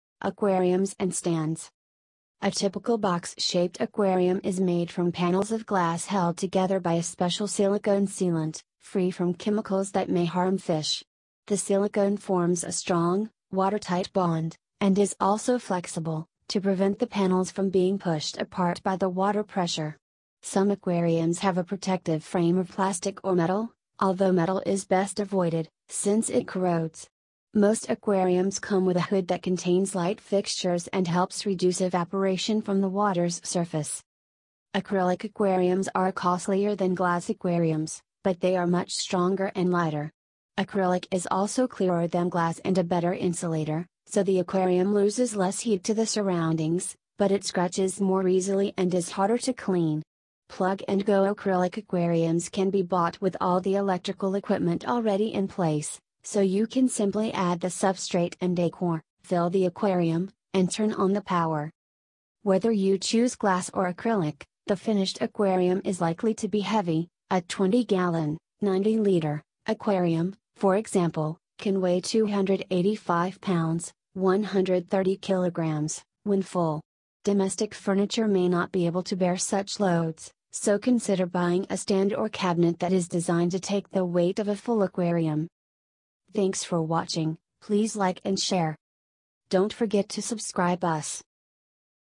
Aquarium & Stands Part 1
Whether you choose glass or acrylic, the finished Aquarium is likely to be heavy!